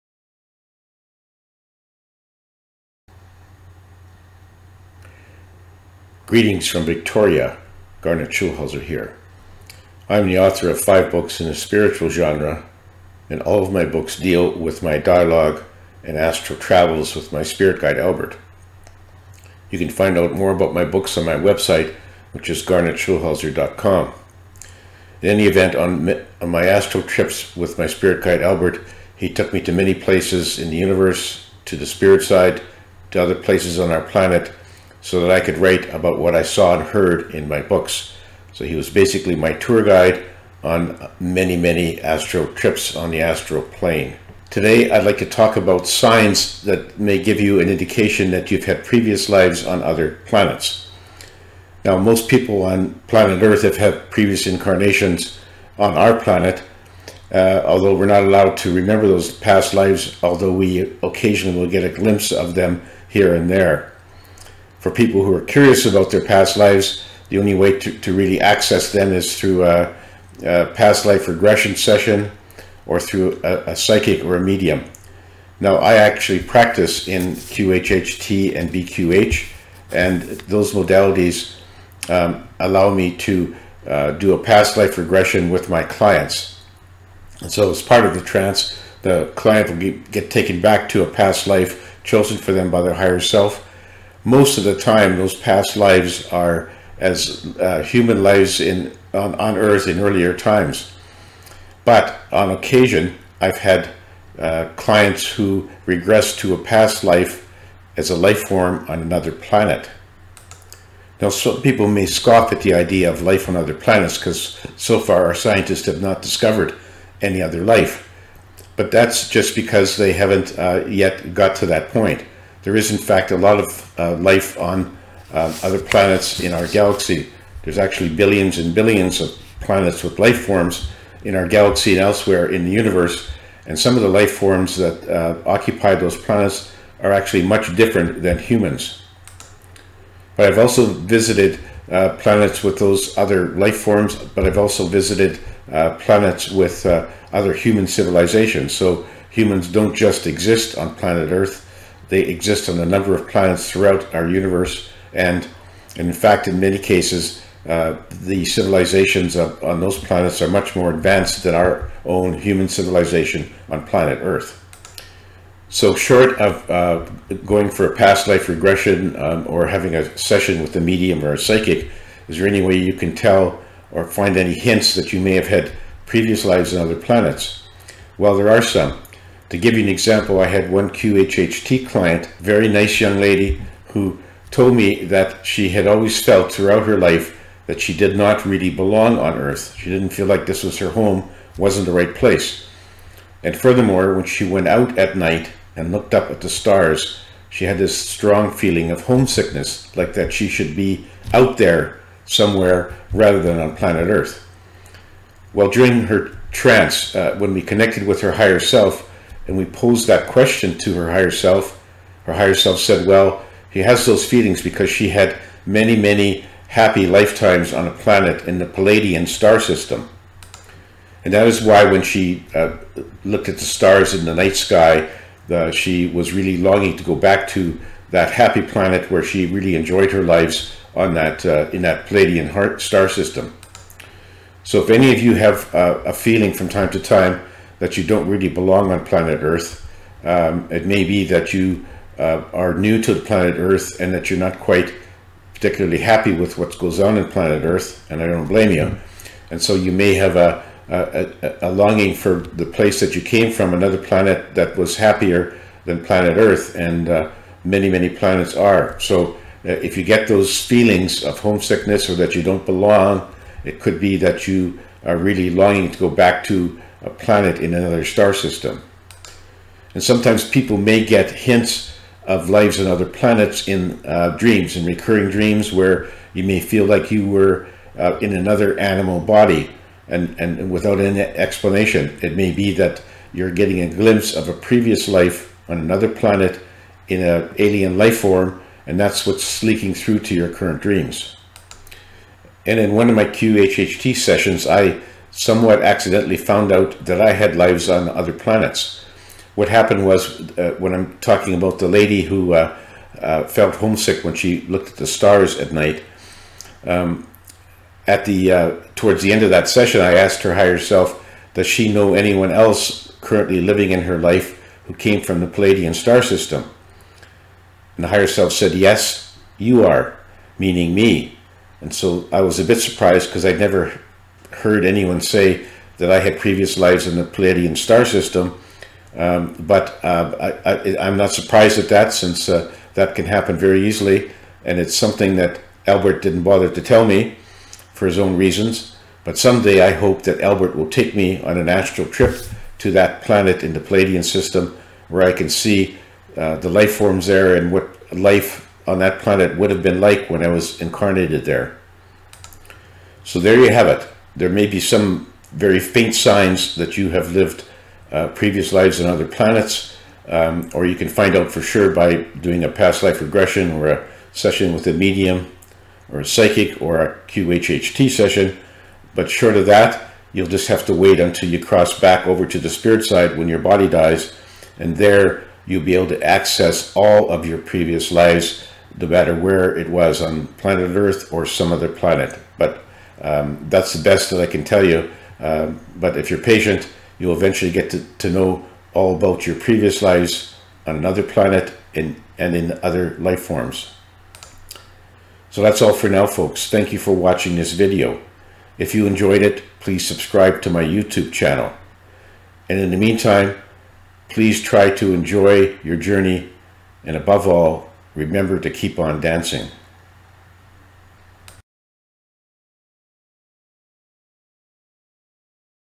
Have You Experienced Past Lives on Other Planets?
Garnet Schulhauser discusses signs that may indicate you have lived previous lives on other planets.